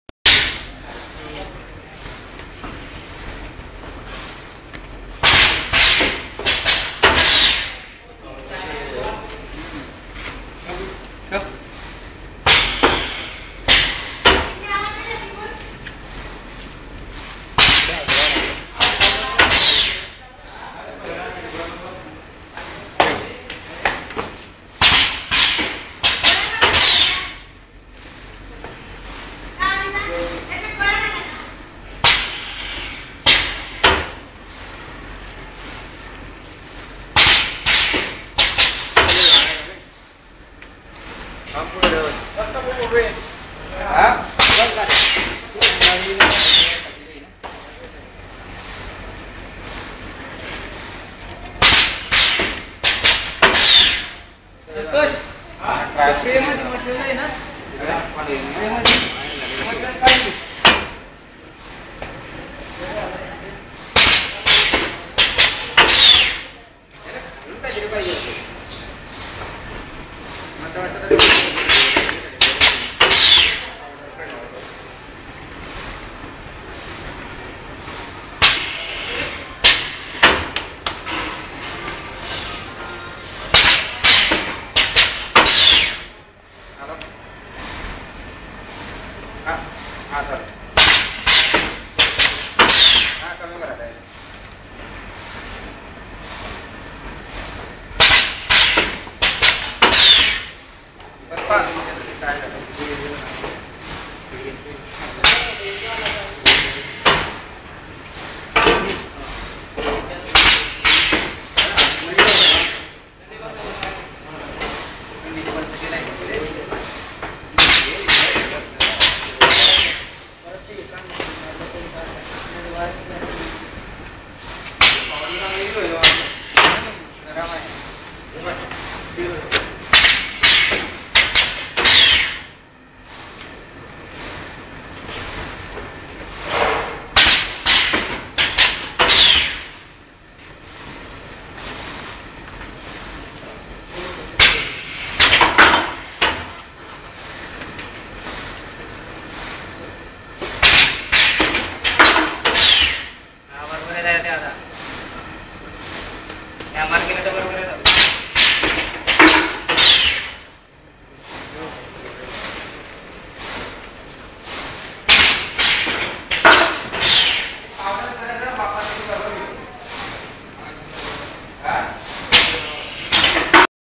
starch moulding jelly candy line
semi-automatic starch  moulding machine line
can make jelly candy, mylikes.
skype:tgmachine.com08